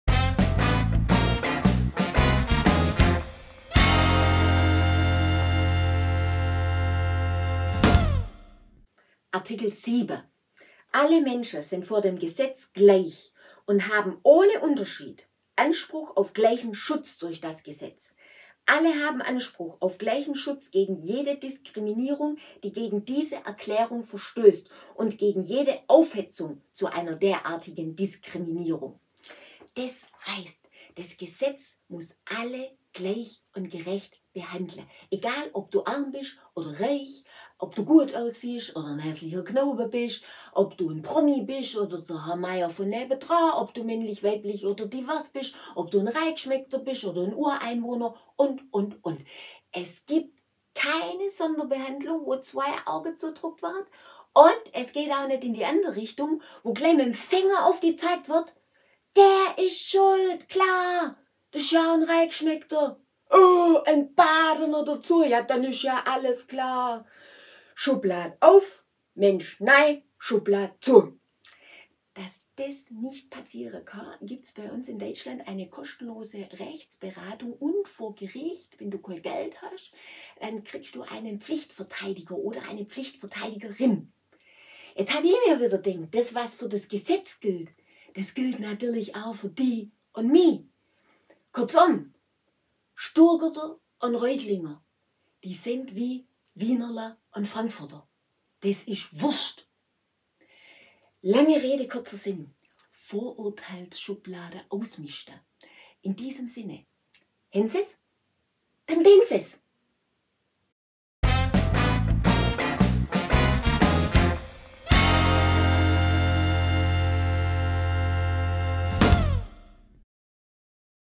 Die 30 Menschenrechte der UN auf schwäbisch: Folge 12 - Das Recht auf Privatleben
Marlies Blume erklärt nach bestem Wissen und Gewissen die 30 Menschenrechte der UN auf gut schwäbisch, damit jeder versteht, um was es geht. Die Allgemeine Erklärung der Menschenrechte, kurz AEMR, erfolgte 1948. Es war die Antwort auf den 2. Weltkrieg mit dem Gedanken: So etwas darf nie wieder passieren! Die Menschenrechte sind global ausgehandelte grundmenschliche Leitlinien, die die freie Entfaltung der Persönlichkeit und ein weltweites Miteinander in Frieden, Freiheit und Sicherheit und ermöglichen sollen. Wir haben allerdings nicht nur Rechte, sondern auch Pflichten. Wir alle tragen Verantwortung für das Große Ganze. Es geht nicht allein um mich, es geht um das Wohl des Ganzen. Meine Rechte enden immer dann, wenn ich die Rechte anderer verletze. Die Menschenrechte sind somit nicht nur ein Aufruf an die Staaten, sondern auch an jeden von uns. Das heißt, wir alle sind dazu aufgerufen, die Menschenrechte zu leben und zu verwirklichen. Große Veränderungen beginnen immer im Kleinen. In diesem Sinne: Hend Se’s? Dann dend Se’s! Oder: Haben Sie es verstanden? Dann handeln Sie entsprechend!

Das Projekt wurde gefördert durch ein Stipendium des Ministeriums für Wissenschaft, Forschung und Kunst Baden-Württemberg